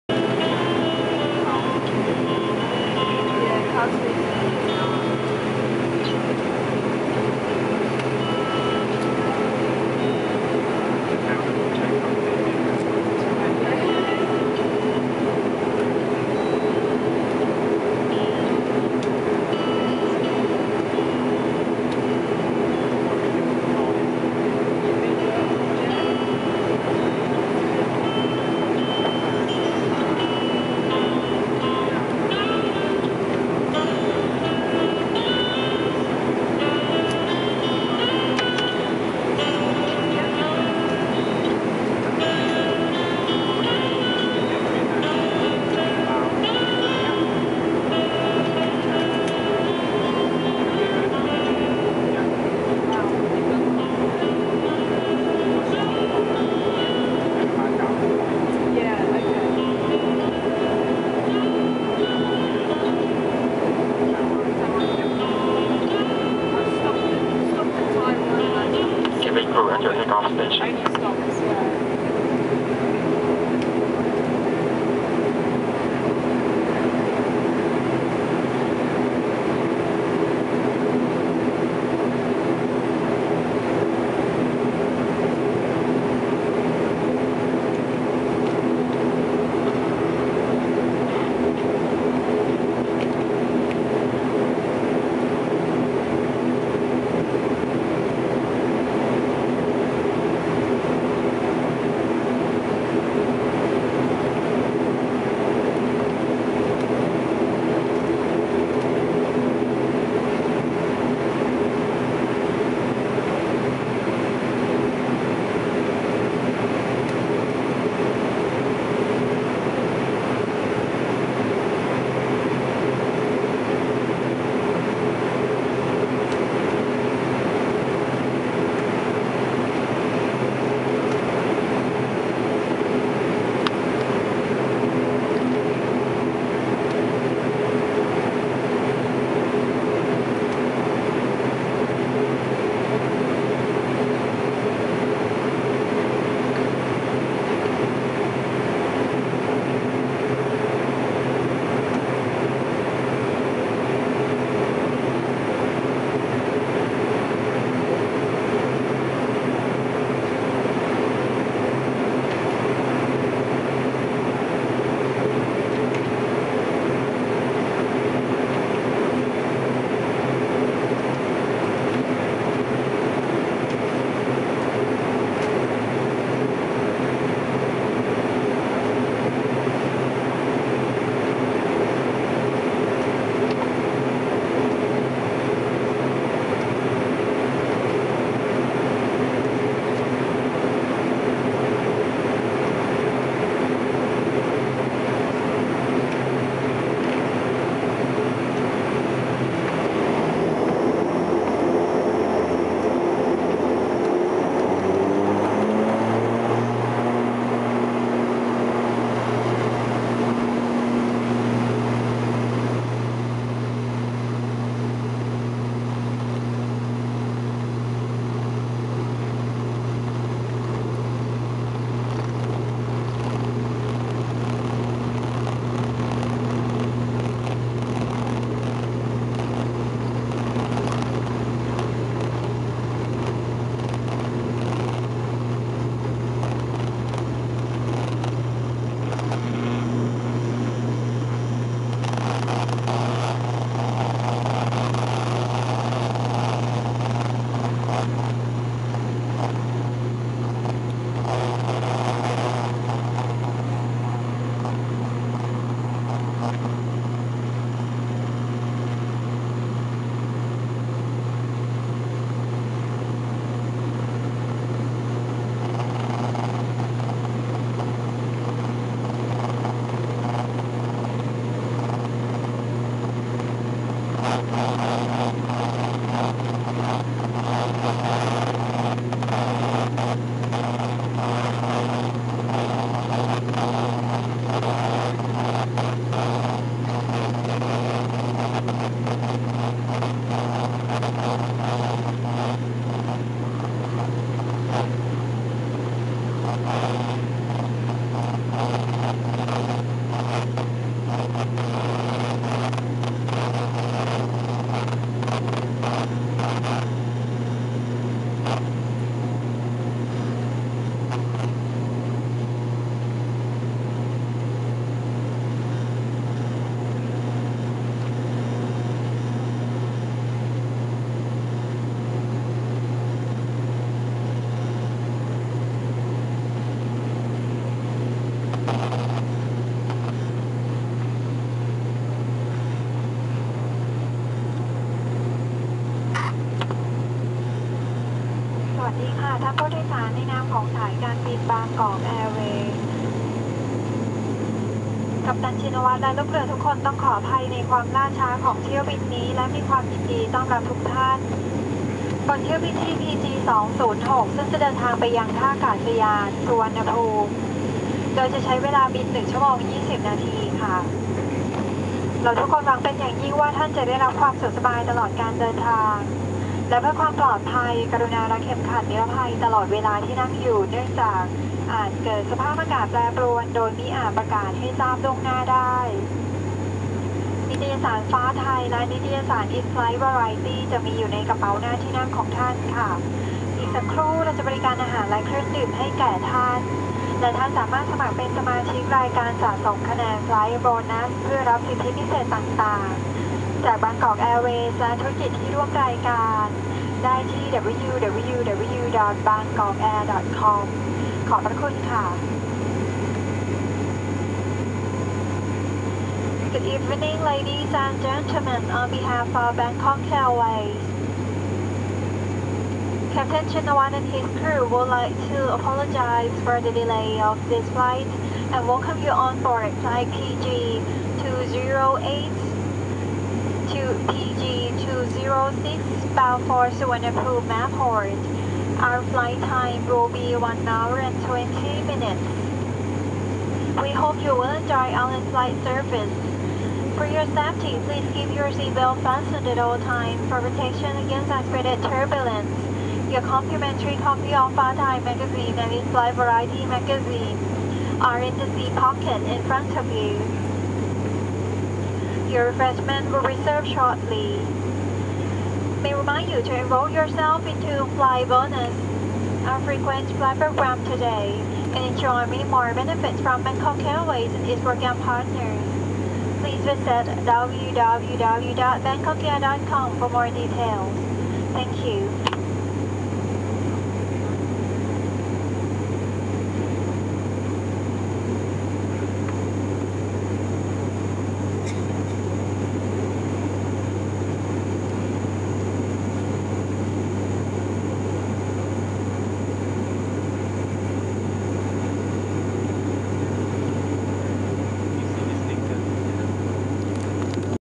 Take off at Lampang Airport (Lampang-Thailand)
Bangkok Airways ATR72-500 HS-PGG Flight PG206 Take off at Lampang Airport (LPT) to Suvarnabhumi Airport (BKK) on July 24th, 2013 at 1820